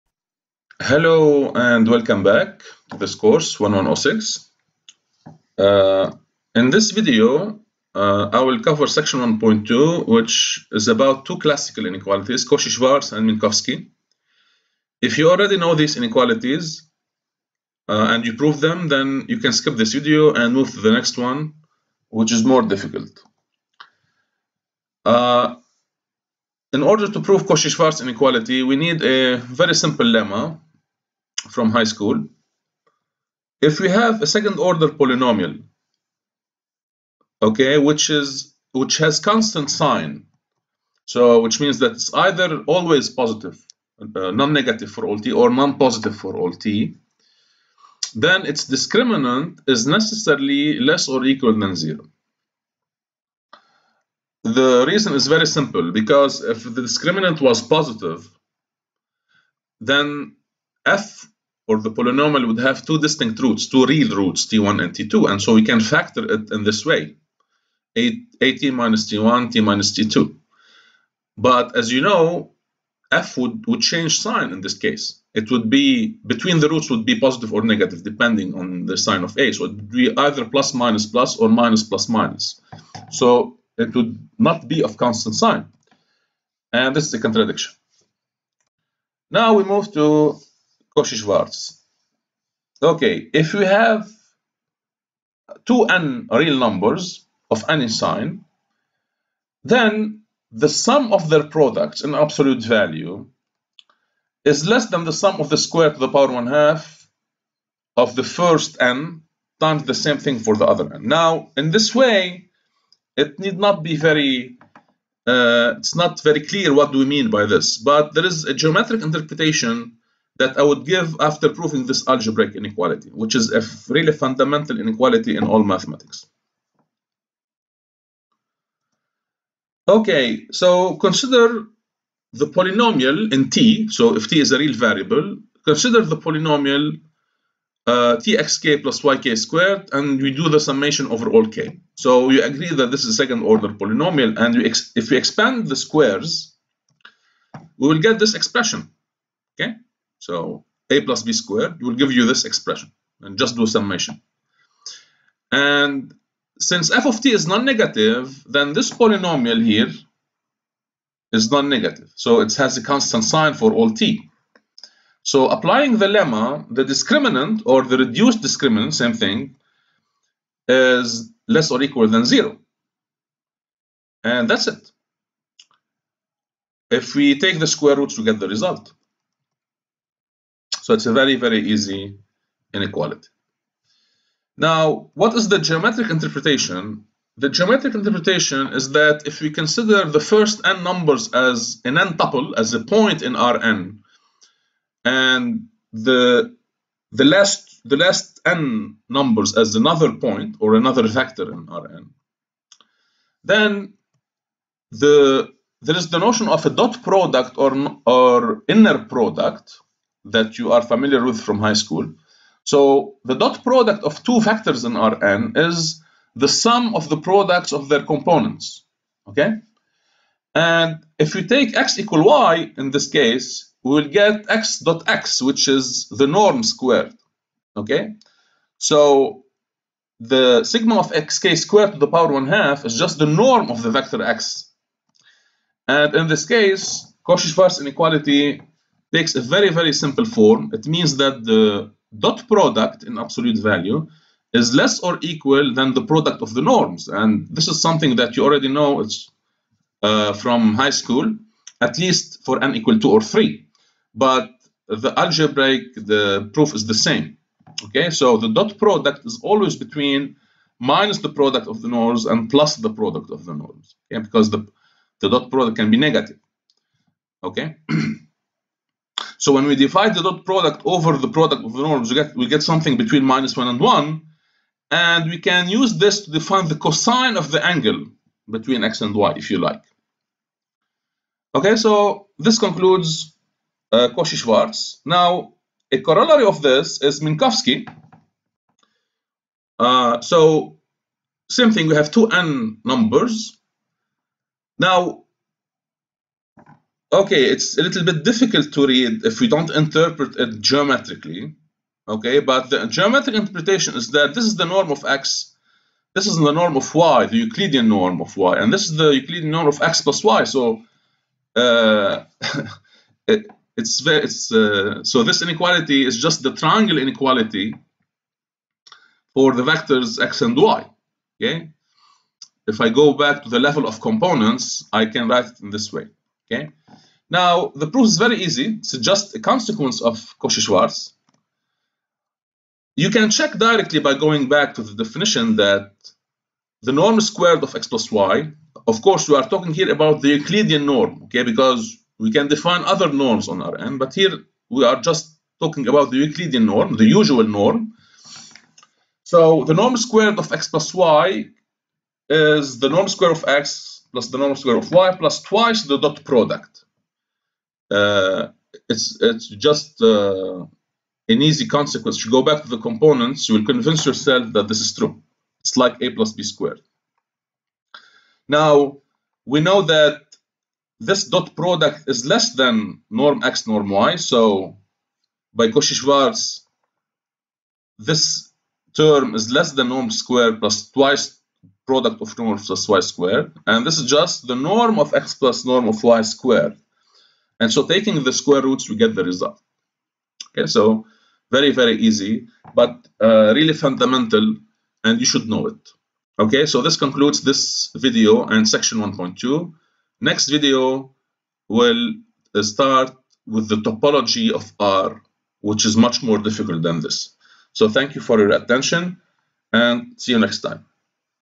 M1106 - Analysis IV - Section 1.2 - Two classical inequalities
Cauchy-Schwarz and Minkowski's inequalities